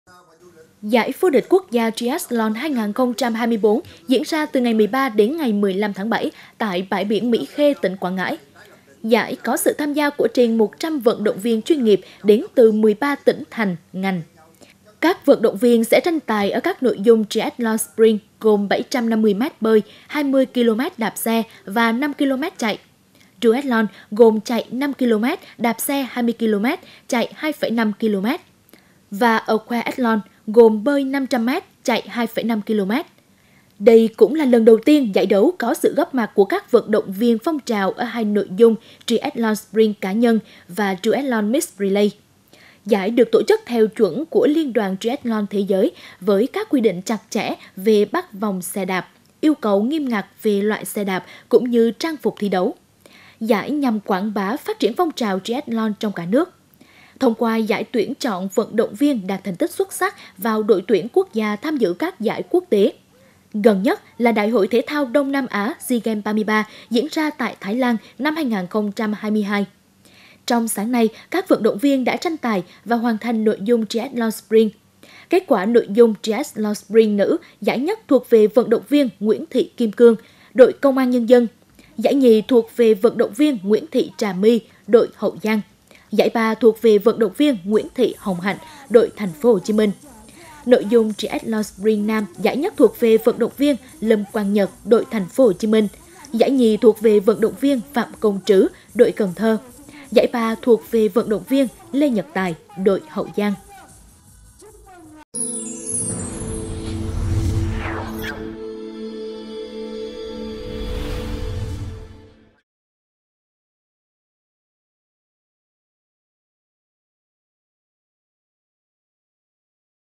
100 vận động viên tranh tài Giải vô địch quốc gia Triathlon 2024 tại Quảng Ngãi | PTQ
Sáng nay (13/7), Sở Văn hóa, Thể thao và Du lịch tỉnh phối hợp với Cục Thể dục thể thao và Liên đoàn Triathlon Việt Nam tổ chức Lễ Khai mạc Giải vô địch Triathlon quốc gia năm 2024. Đây là giải Triathlon quốc gia đầu tiên được tổ chức tại Quảng Ngãi.